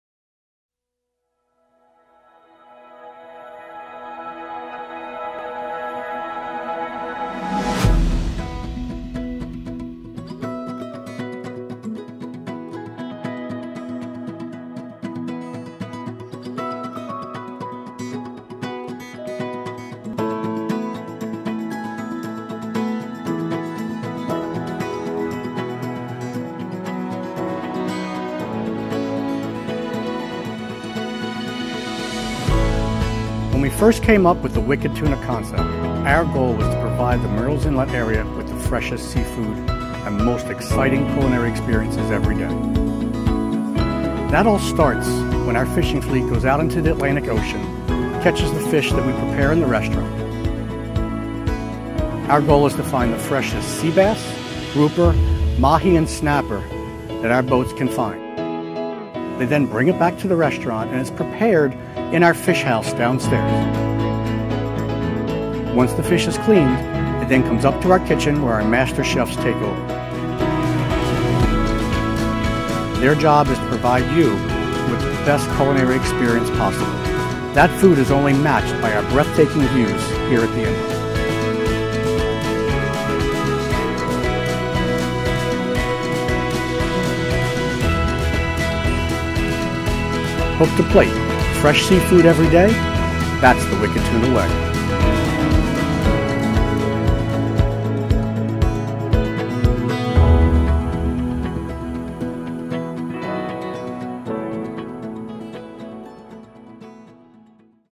Wicked Tuna "Hook To Plate"
Wicked Tuna's "Hook to Plate" fresh every day  seafood and sushi philosophy.